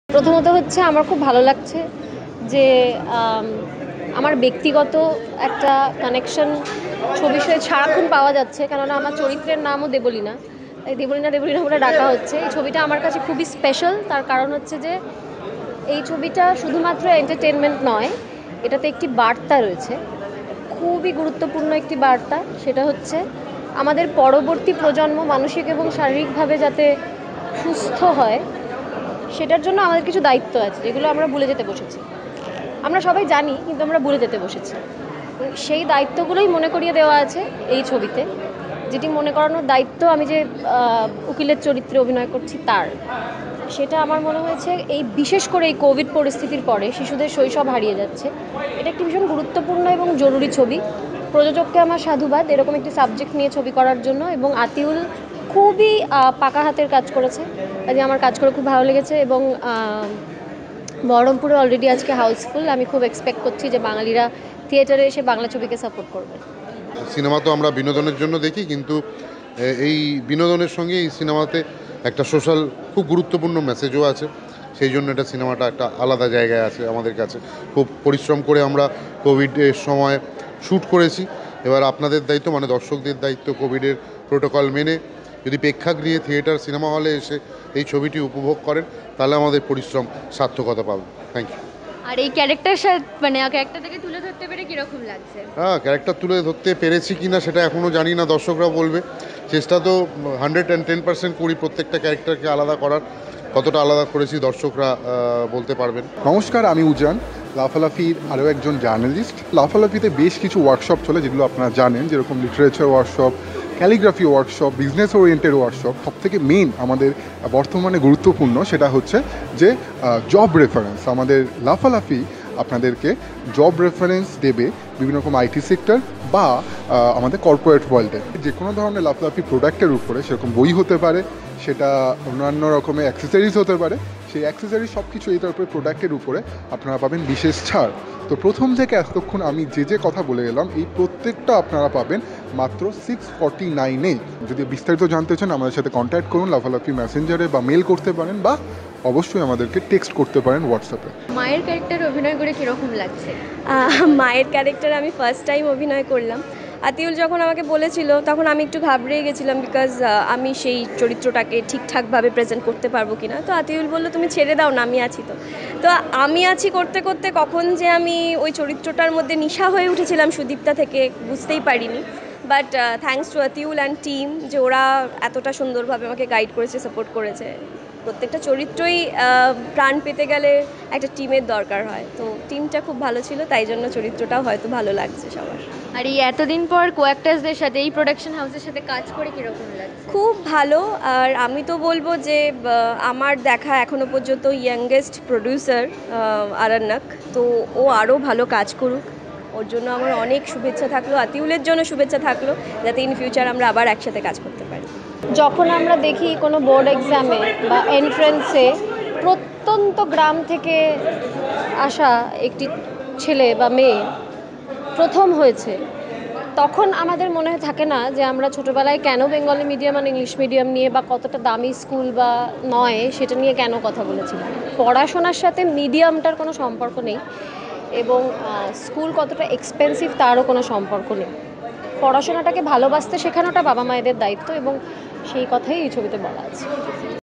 Kishalay Movie Premeire | Debolina Dutta | Aishwarya | Ujan | LaughaLaughi
The story revolves around a 7-year-old girl who committed suicide while no one was at home.

অরণ্যক ফিল্মস প্রযোজিত নতুন বাংলা সিনেমা ' কিশলয় '।

সিনেমাটি পরিচালনা করেছেন  'আতিউল ইসলাম'  এবং  সিনেমাটি তৈরি হয়েছে 'তানভীর কাজী'-র গল্প অবলম্বনে।

'কিশলয়'  সিনেমাটি  উজ্জ্বলভাবে বর্তমান সমাজের একটি জ্বলন্ত বিষয়কে তুলে ধরেছে,
এই সিনেমার ধারণাটি খুবই গুরুত্বপূর্ণ যেটি,  প্রাসঙ্গিক এবং সমাজের কঠোর সত্য।।

Debolina Dutta | Sudip Mukherjee | Sudipta Chakraborty | Vivek Tribedi | Debraj Mukhopadhyay | Adrija Mukhopadhyay

Journalist: Aishwarya Manna
Photo Journalist: Ujan Guha